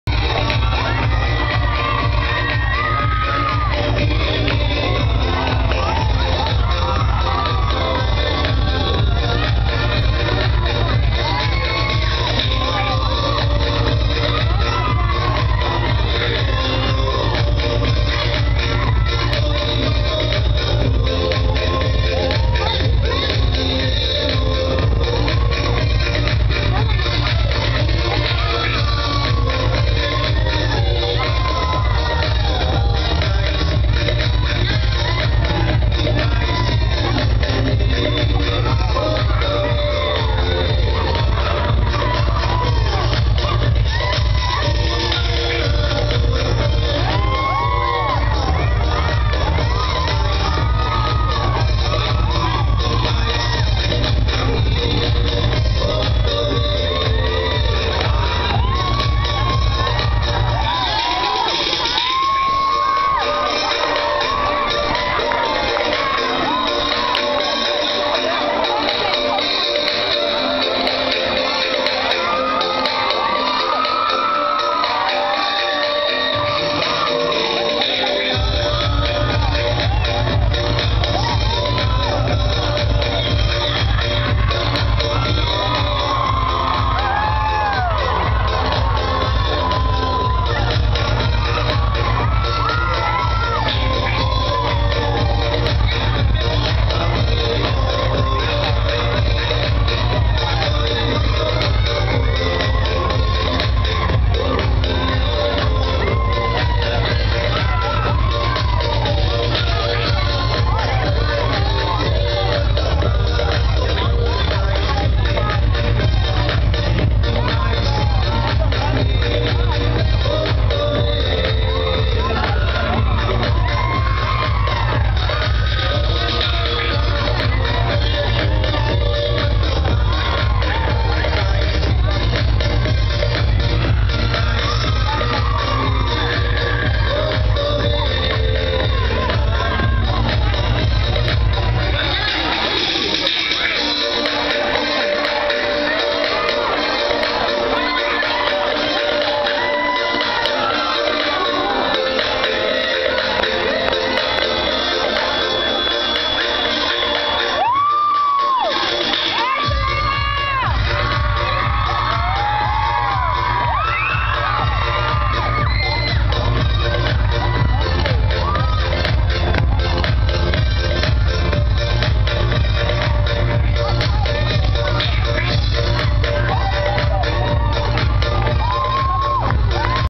Tongan Dance (BGHS 50TH ANNIVERSARY)
This was a tongan performance i did with some of my friends on the Anniversery of our school.